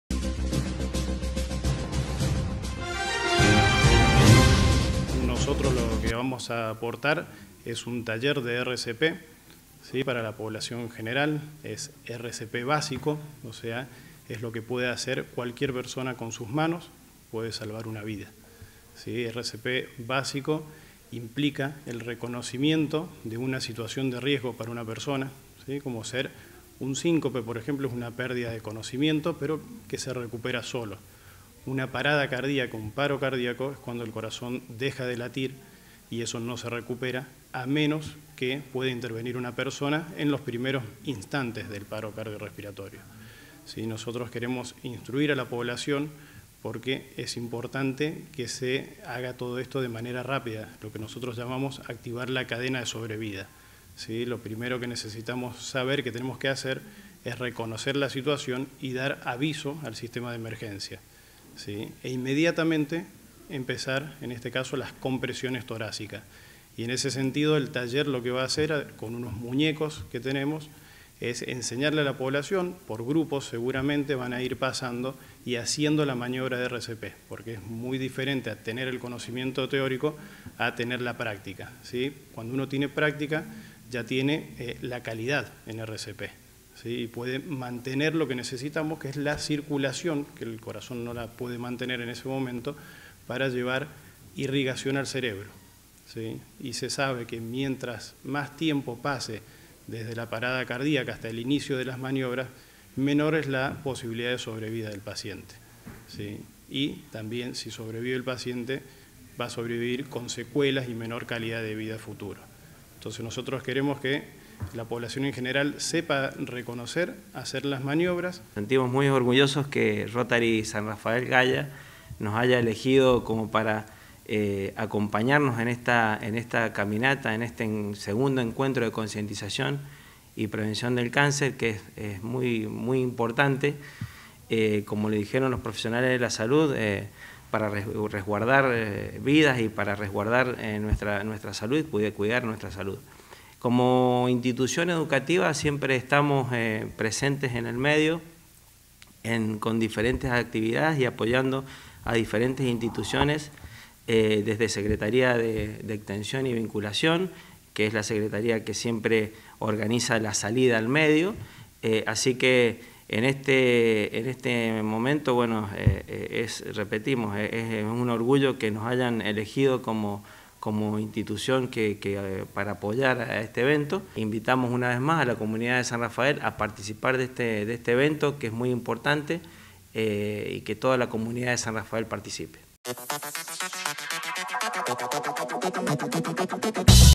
PASO A PASO POR LA SALUD
El Dr. Leonardo Schiavone, Presidente de la Sociedad Cardiológica del Atuel y Rogelio Oscar Di Santo, Vicedecano de la Facultad de Ciencias Aplicadas a la Industria, realizarán junto al Rotary Club una caminata este próximo fin de semana, en la Plaza Francia.